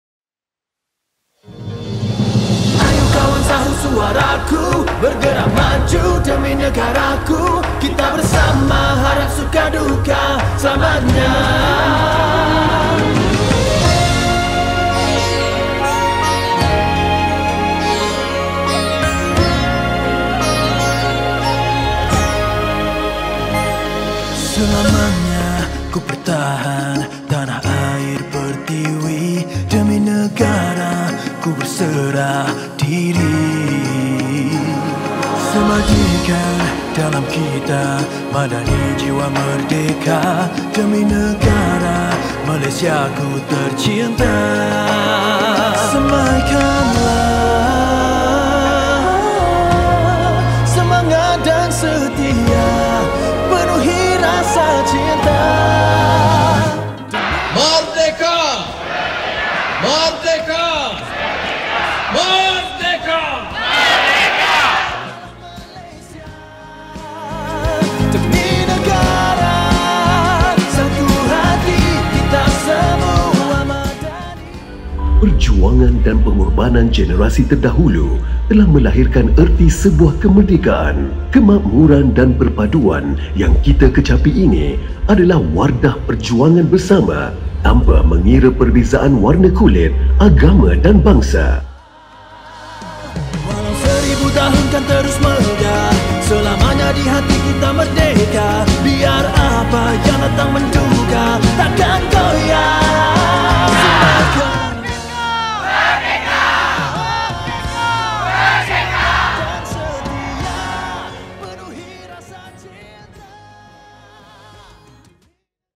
30.08.2024 - MAJLIS AMANAT PERDANA HARI KEBANGSAAN 2024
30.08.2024 | YB Dato' Haji Mohammad Yusof Apdal, Timbalan Menteri Sains, Teknologi dan Inovasi; YBhg. Dato' Ts. Dr. Haji Aminuddin bin Hassim, Ketua Setiausaha MOSTI, barisan pengurusan tertinggi MOSTI, jabatan dan agensi serta warga MOSTI turut bersama lebih 7,000 tetamu yang hadir semasa Majlis Amanat Perdana Hari Kebangsaan 2024 di Pusat Konvensyen Antarabangsa Putrajaya (PICC).

YAB Perdana Menteri dalam ucapannya mengajak seluruh warga Malaysia menghayati erti kemerdekaan dengan semangat Jiwa Merdeka, mengekalkan keharmonian bangsa serta sentiasa bersatu padu dalam membina negara.

Ucapan YAB Perdana Menteri di akhiri dengan gesaan karyawan yang juga sasterawan negara, saudara A. Samad Said “Anak-anak kita bakal datang tangkas dan berilmu; dan kita, dalam sejarah, bakal syahdu dan bangga, kerana awan yang indah, lapis pelangi lebih mesra, wajah diri dan inti peribadi kita tunjang bagi kehidupan dan kemerdekaan bangsa yang sentiasa sedia belajar mengerti dan tekun hidup bersama.”

MERDEKA! MERDEKA! MERDEKA! MERDEKA!